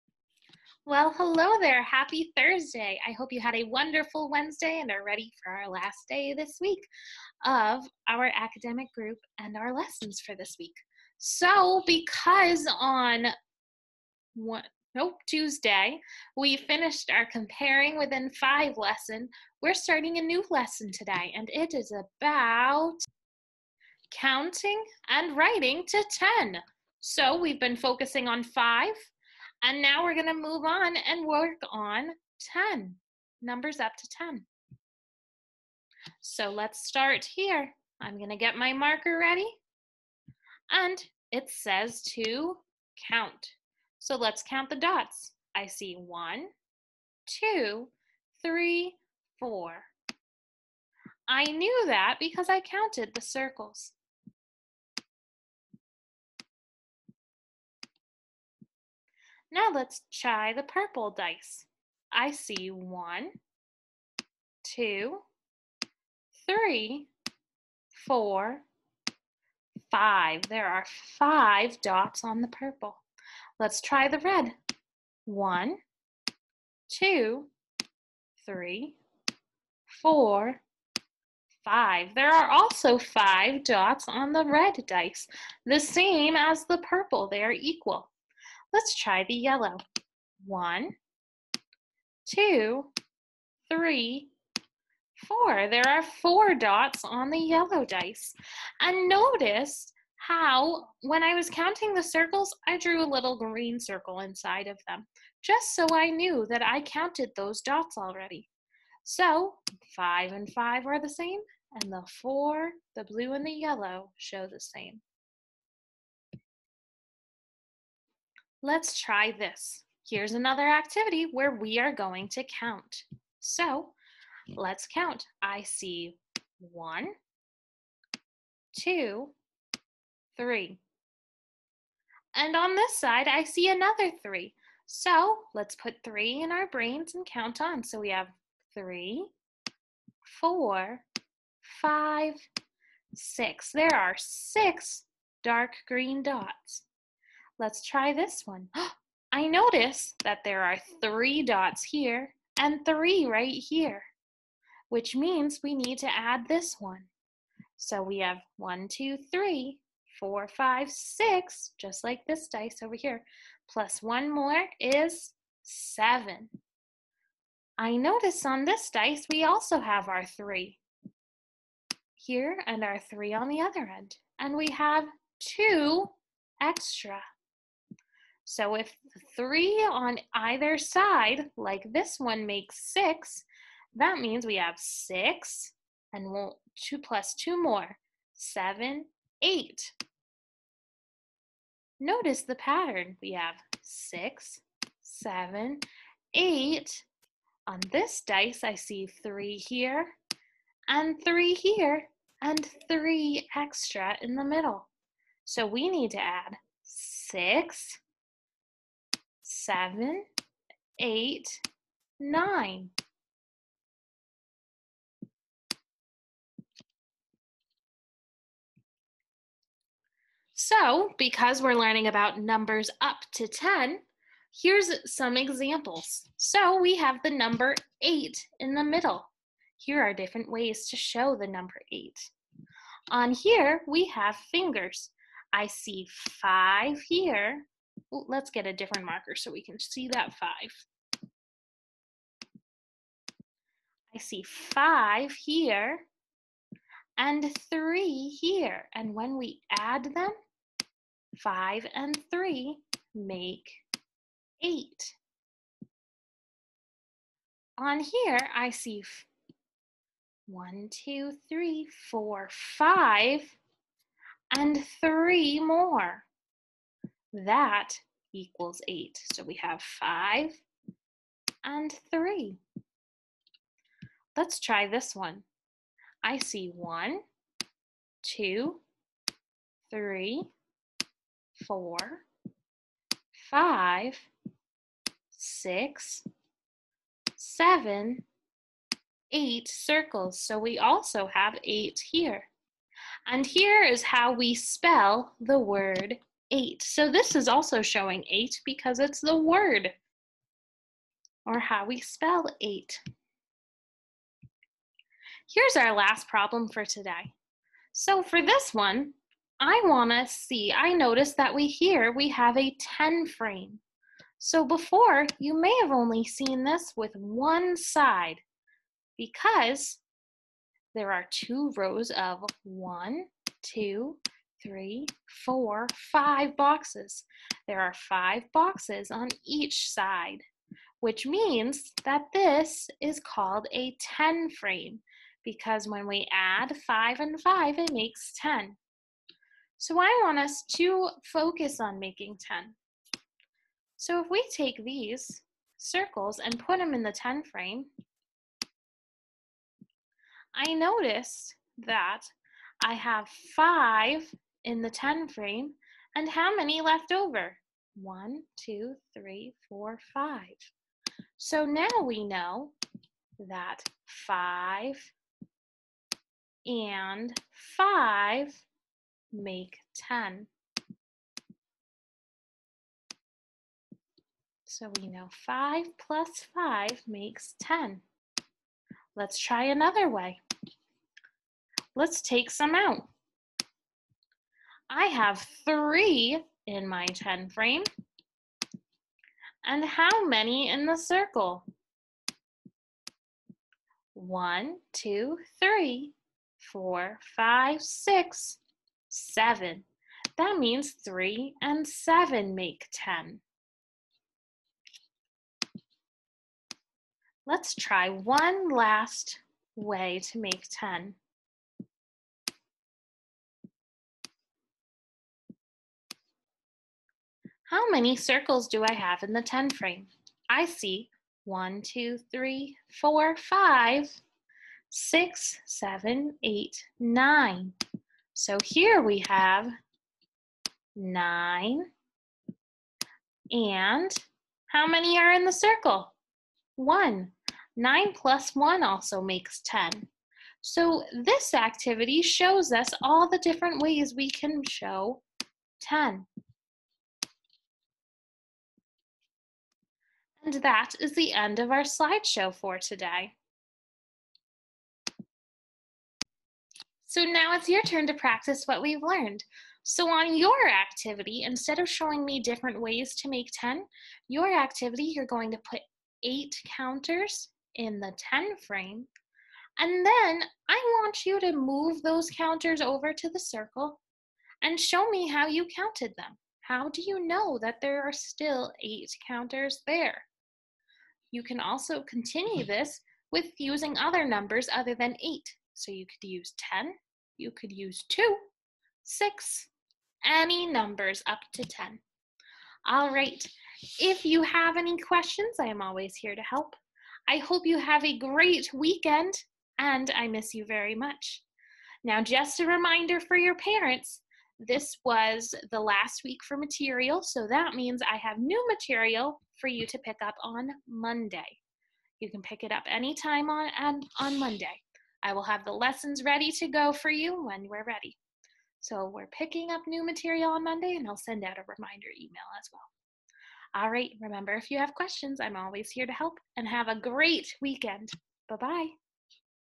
Count & Write to 10 (part 1)
In this video, students will learn about numbers up to 10.

Kindergarten iReady lesson 6, session 1.